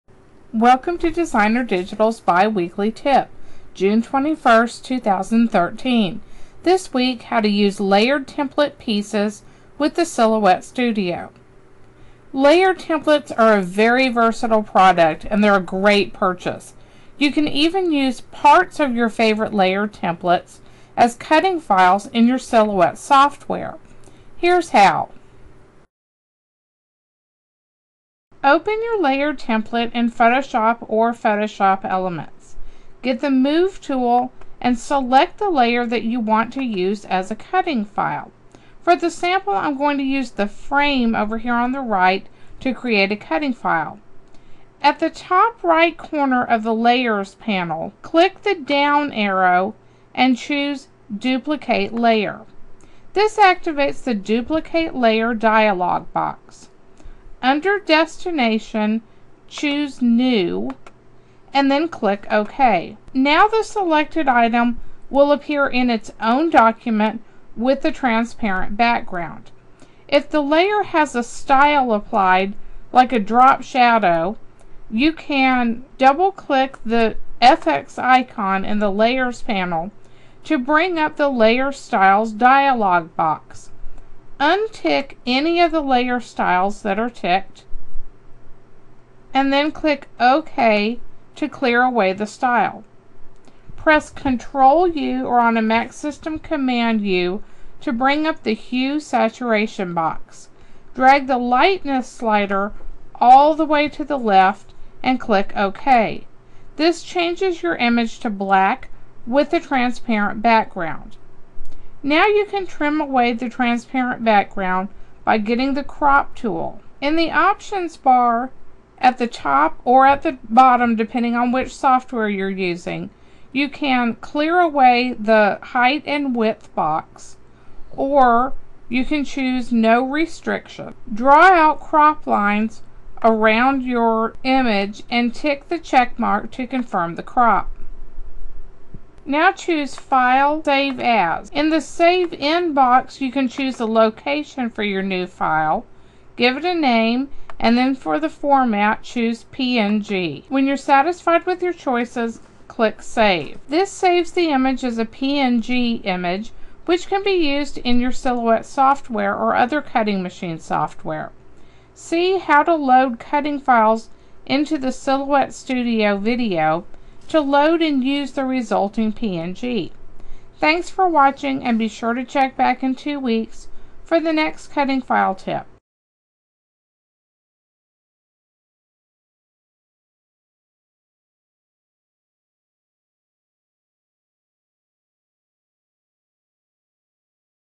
How to Use Layered Template Pieces with the Silhouette Studio
Adobe Photoshop and/or Photoshop Elements or other software Video how-to from DesignerDigitals. This week: How to Use Layered Template Pieces with the Silhouette Studio.